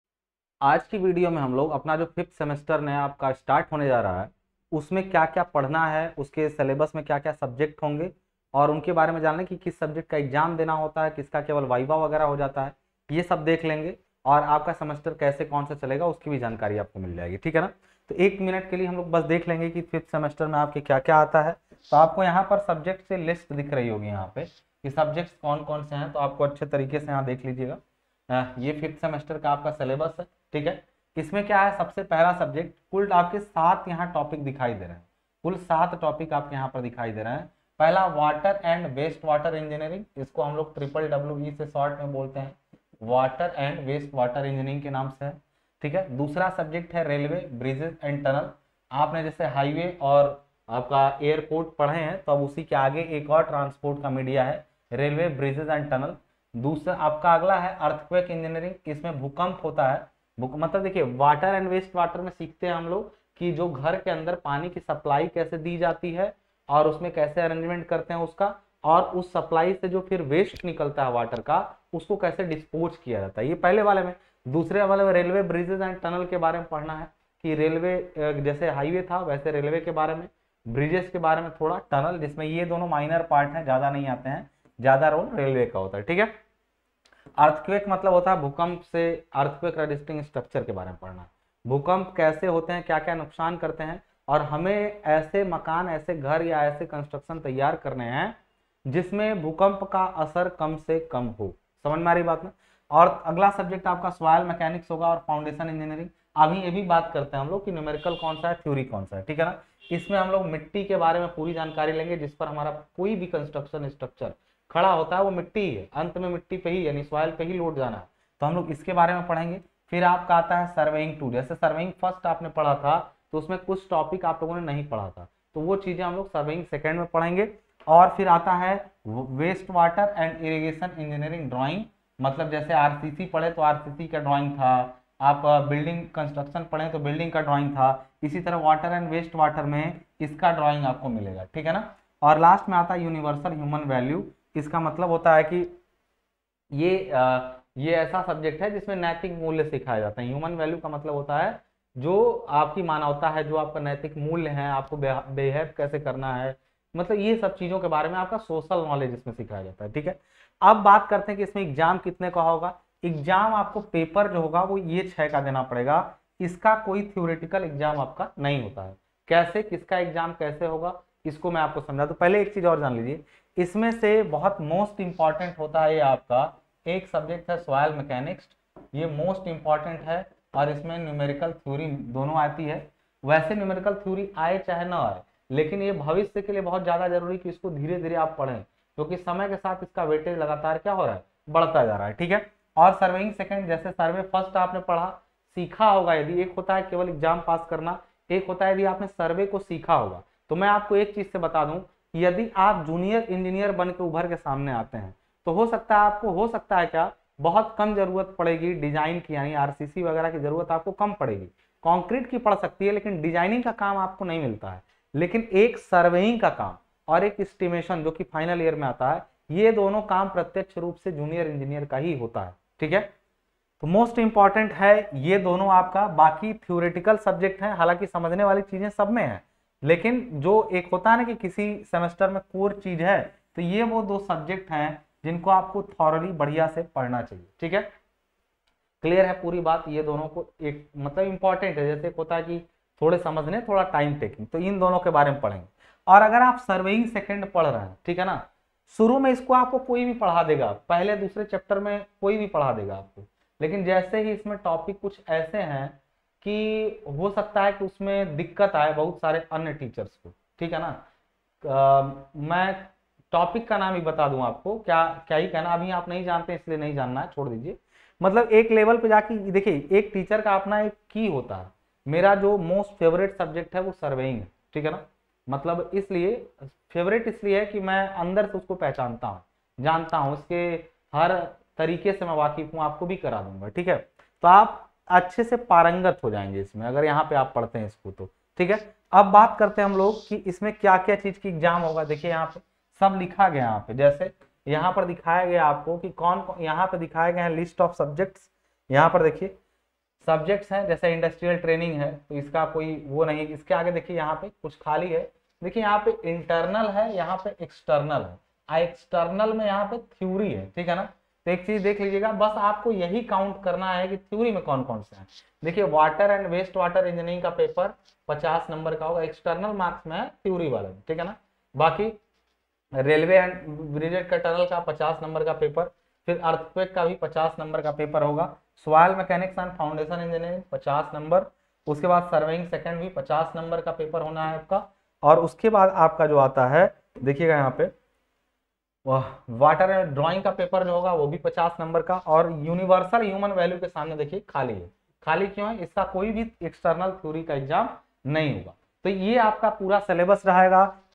BTEUP 5th semester Syllabus- सभी विषयों की जानकारी और pattern Discussion by Ashwini Sharma
Bteup_Syllabus_5th_sem_2022